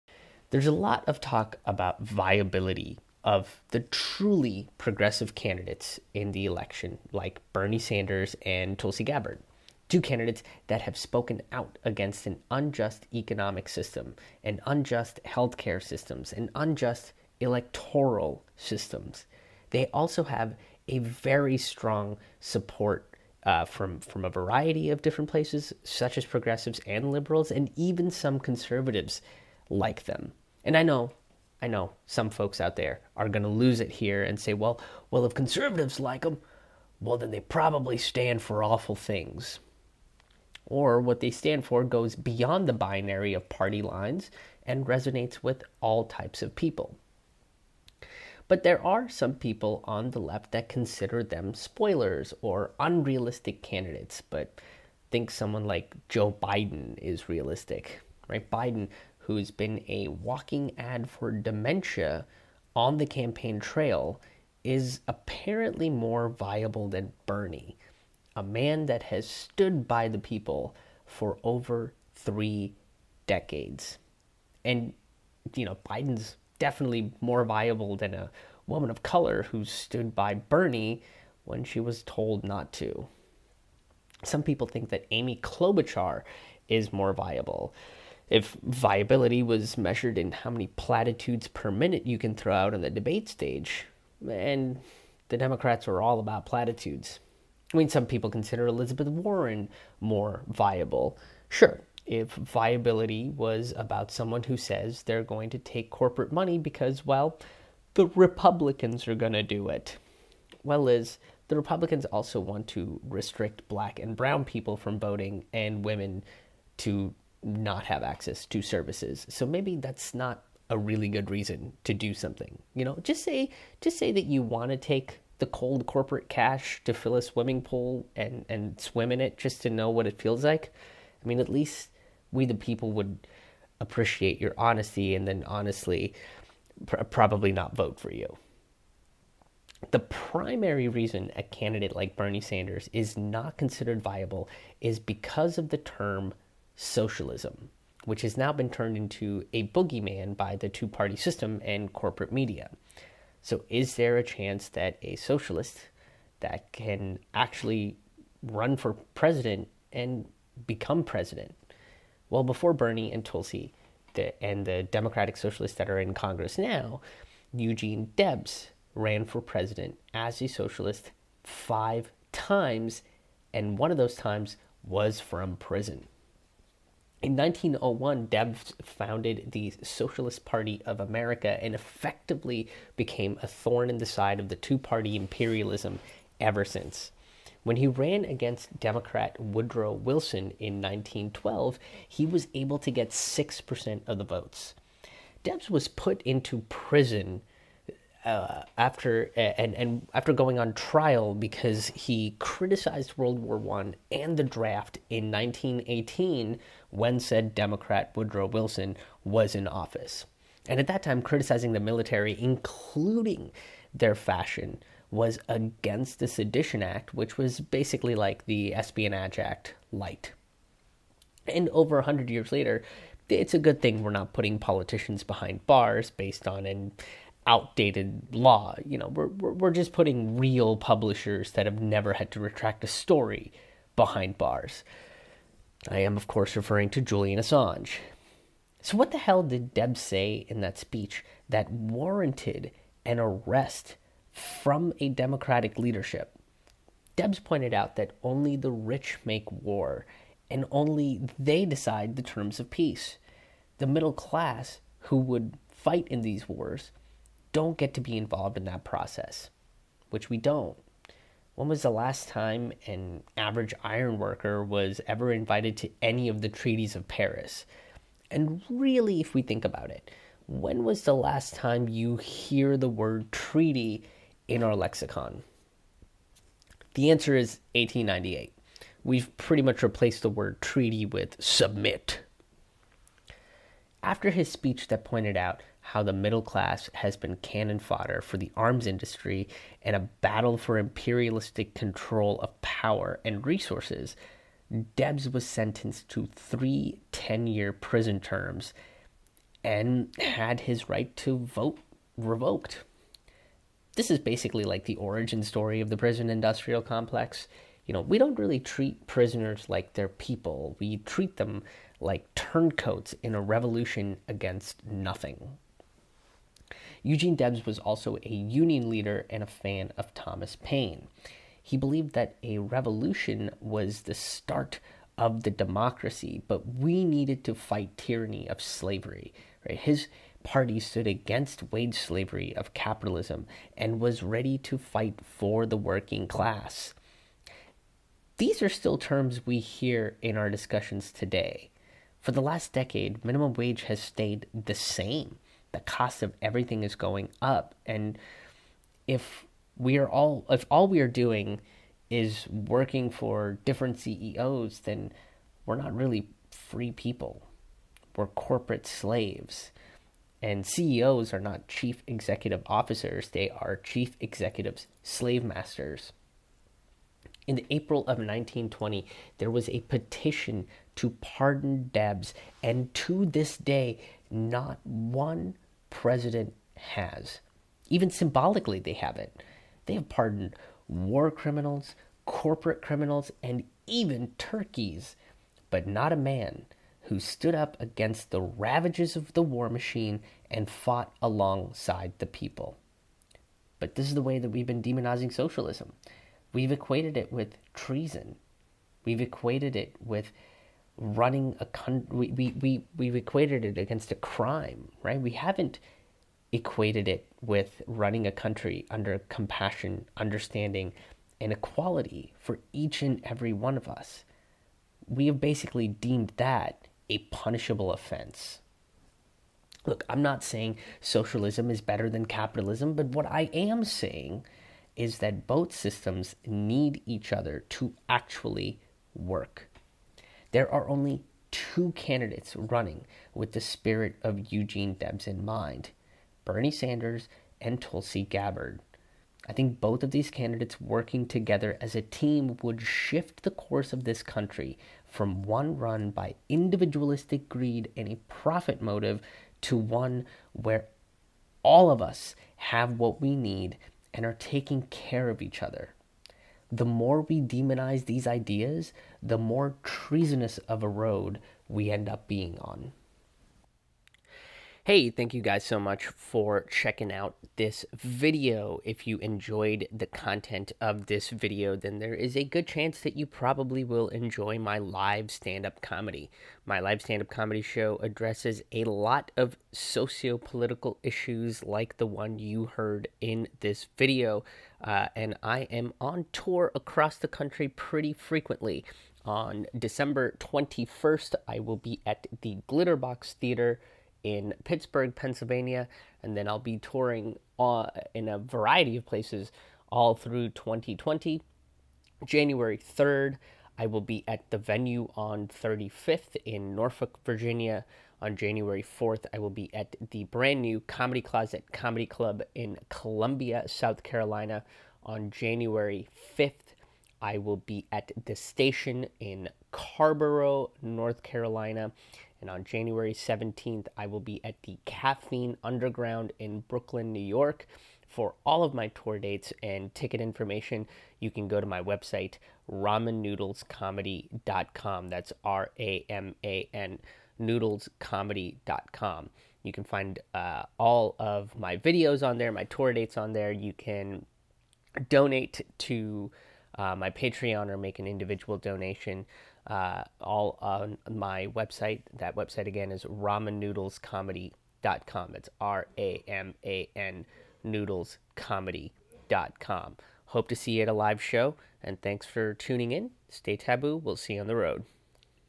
Eugene Debs Ran For President A SOCIALIST! [The Dispatch]
We've talked about whether we are ready for a Socialist President, but we've already had a Socialist Presidential candidate! Eugene Debs was a Socialist, Anti-War President that ran in the early 1900s!

Get her full album here: lissvictory.bandcamp.com